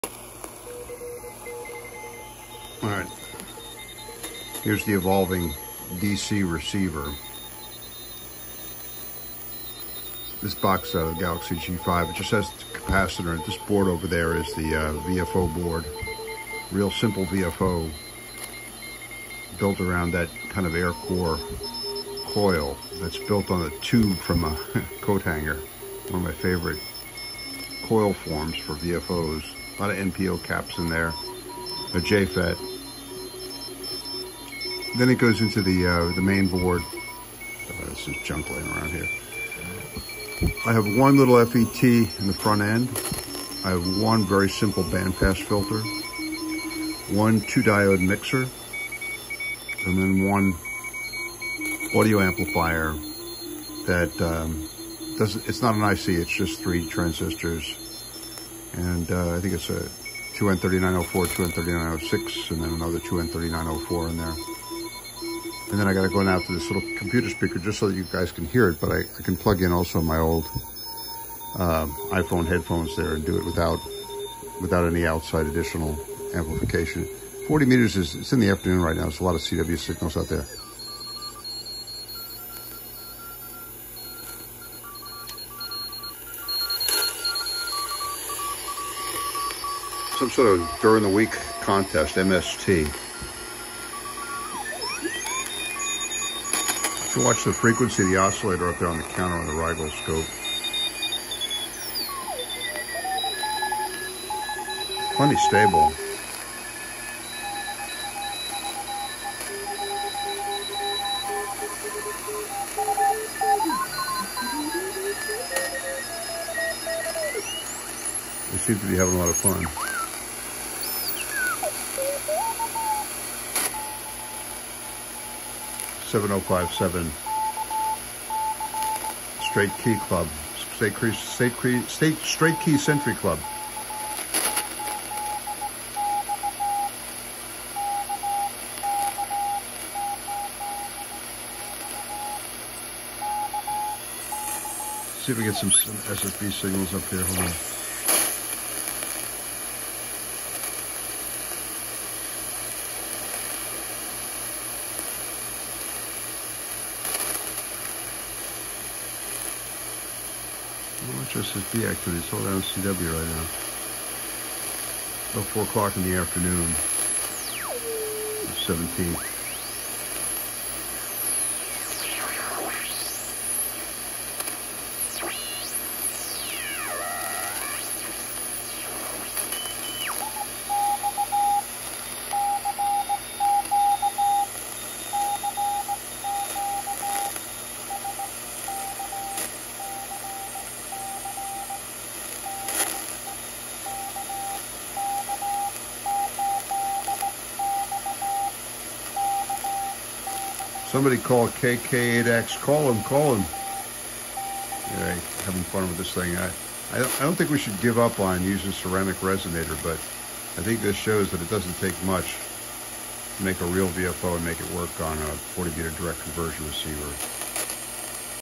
A 40 meter Direct Conversion Receiver with an Analog VFO
This is the DC receiver that I built back in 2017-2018.   I used a ceramic resonator in the VFO.  The receiver was on the cover of SPRAT magazine.  It may not have deserved the honor -- recently Dean KK4DAS and I discovered that the ceramic resonator VFO drifted rather badly.  So Dean and I are now building real LC analog VFOs.  This is part of Virginia Wireless Society's Maker Group project.  This video shows my old receiver working today on 40 using the VFO that was recently thrown together.